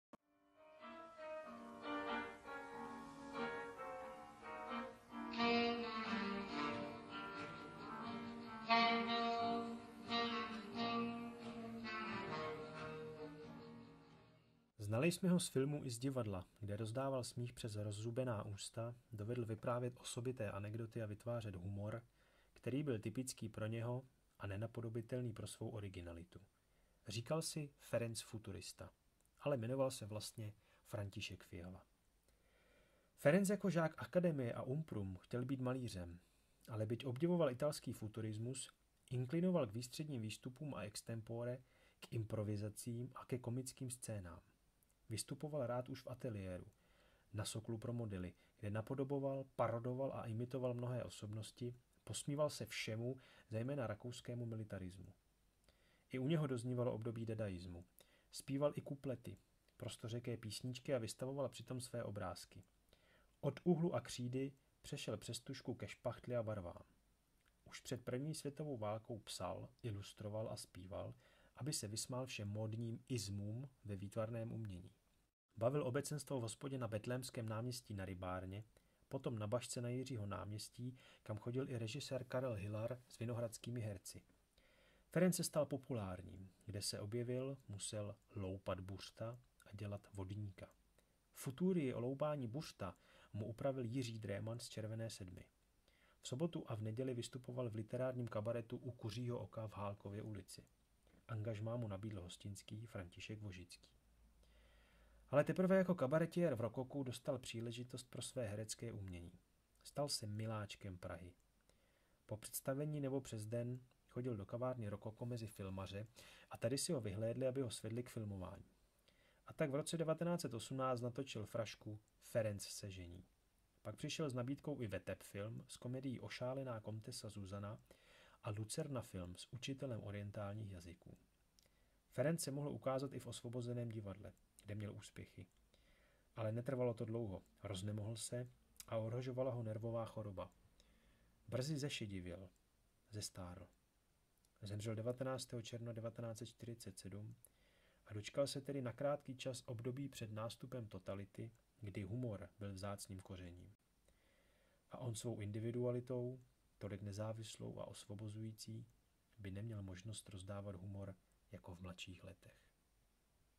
Ferenc Futurista - Kdo to byl ?
FERENC FUTURISTA (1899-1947). Český filmový a divadelní herec a režisér.